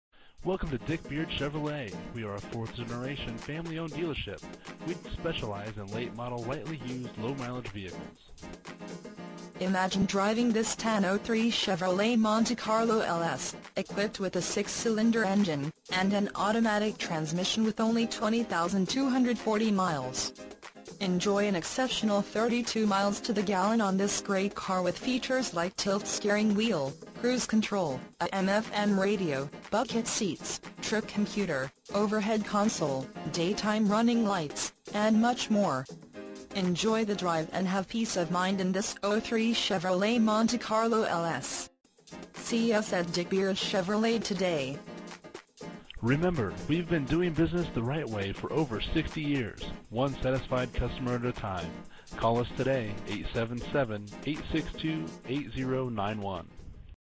SOLD - 2003 Chevrolet Monte Carlo LS 02601 Dick Beard Chevro
Dick Beard Chevrolet

22 Ridgewood Ave.

2003 Chevrolet Monte Carlo LS

 20,240 Miles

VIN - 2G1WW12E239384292

Imagine driving this Tan 2003 Chevrolet Monte Carlo LS, equipped with a 6 Cyl. engine  and an automatic transmission with  only 20,240 miles. Enjoy an exceptional 32 miles to the gallon on this great car with features like Tilt Steering Wheel, Cruise Control, AM/FM Radio, Bucket Seats, Trip Computer, Overhead Console, Daytime Running Lights, and much more. Enjoy the drive and have peace of mind in this 2003 Chevr...